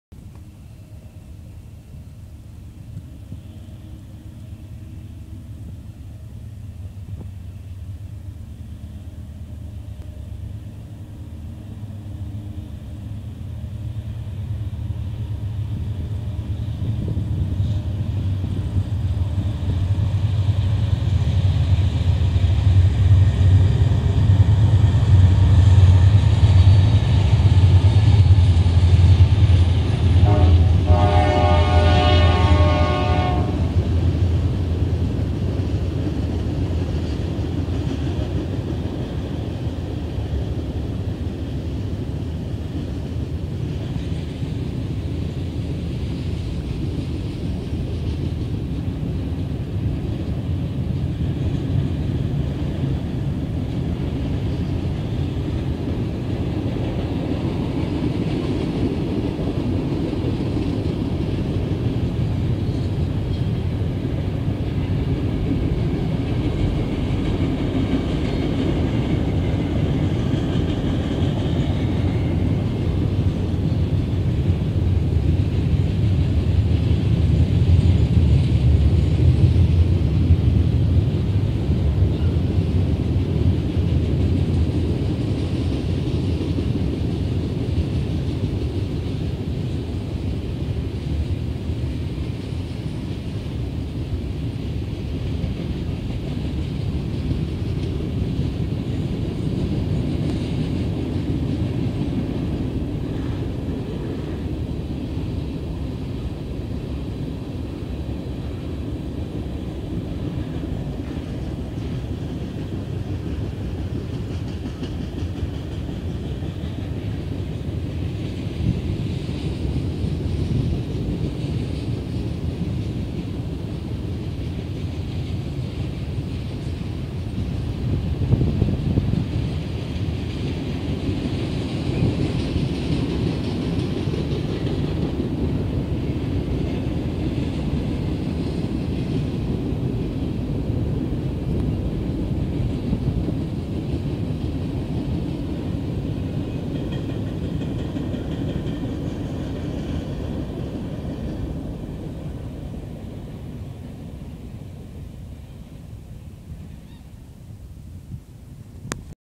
NS 14M East at LSRHS. June 14, 2014
Our next train was NS 14M. Trains on NS seemed to be running behind that day, so we got to see trains we don't normally see. Included in the train were two old "Boston & Maine" hopper cars!